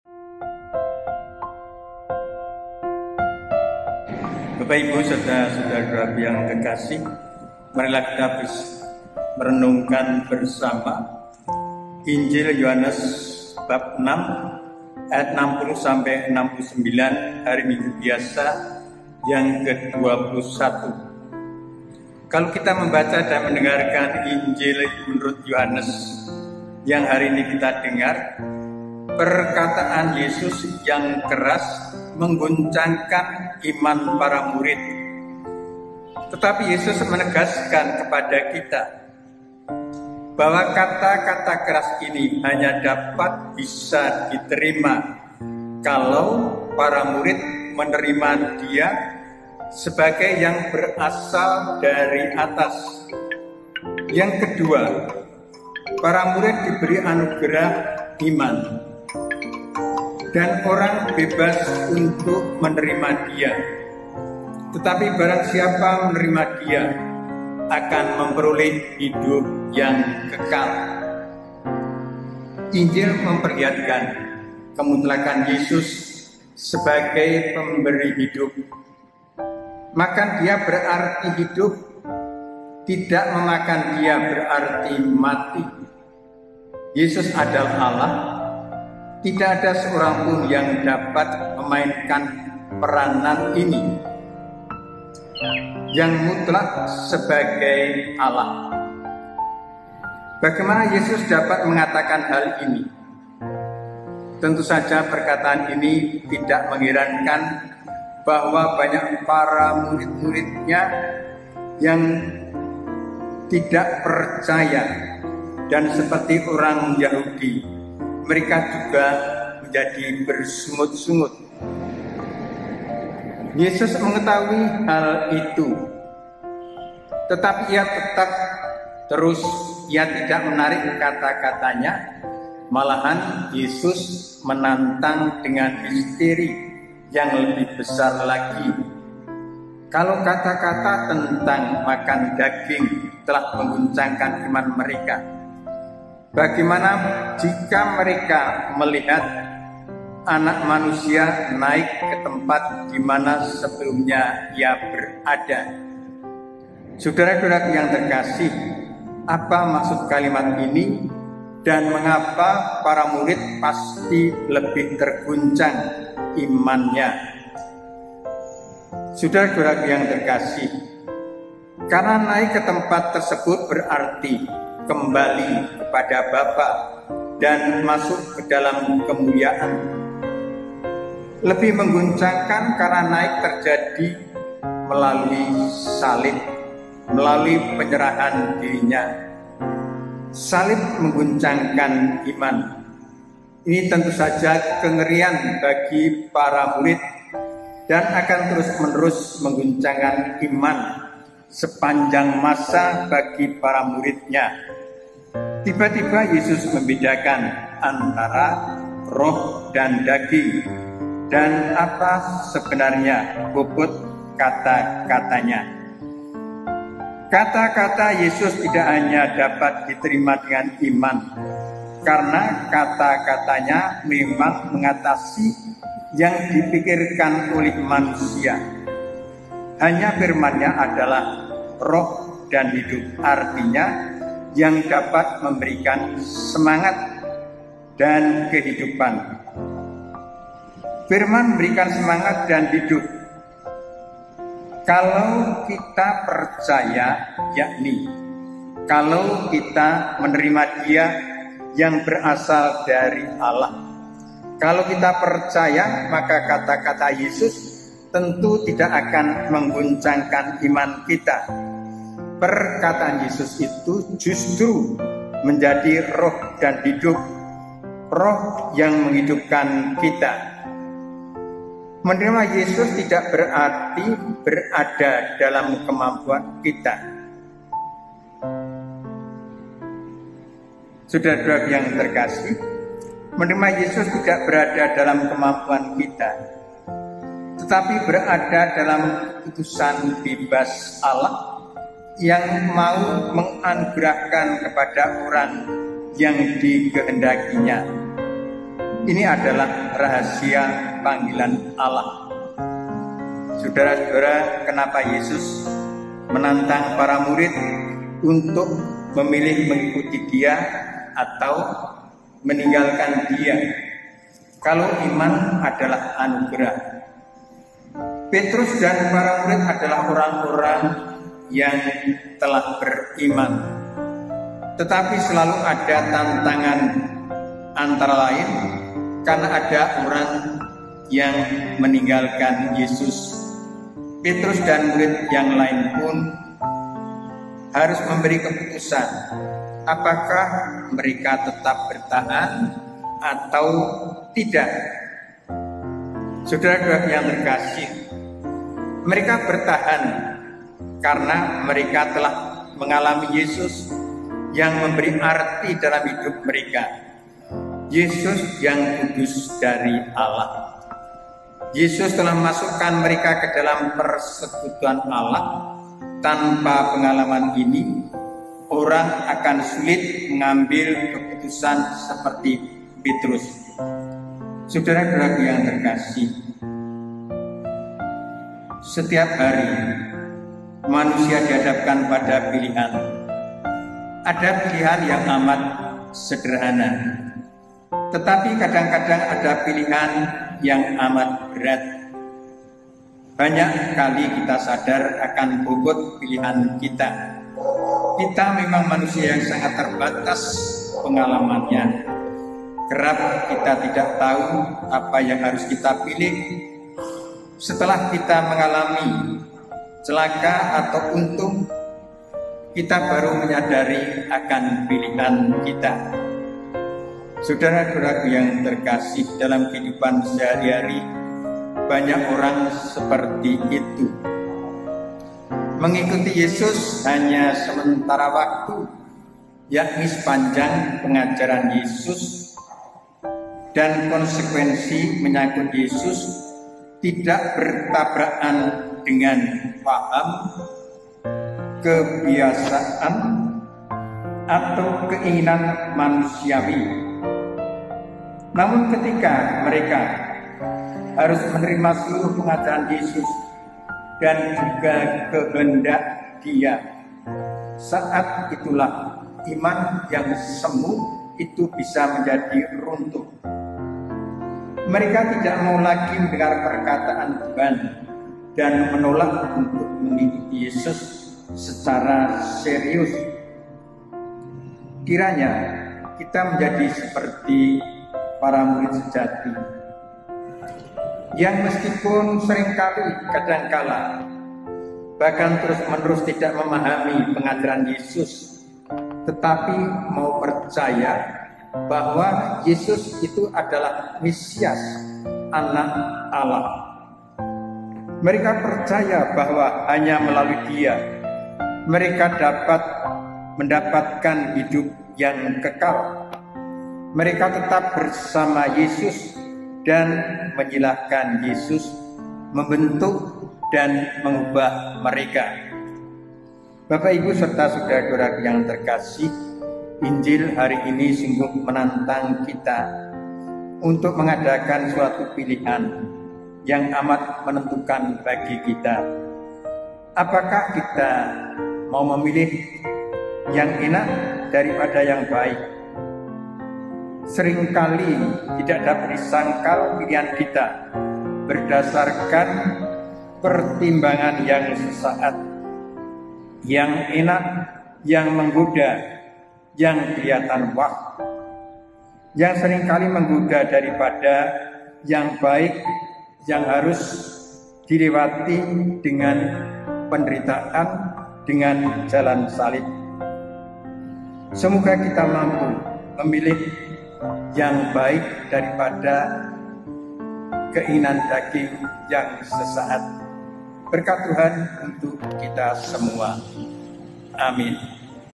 RENUNGAN MINGGUAN: KELUARGA KATOLIK MEMBUAT PILIHAN YANG BENAR DAN TEPAT
Salam dalam Yesus, Maria, dan Yosef. Dalam hidup, kita selalu menghadapi pilihan. Keluarga Katolik ditantang untuk membuat pilihan yang benar dan tepat.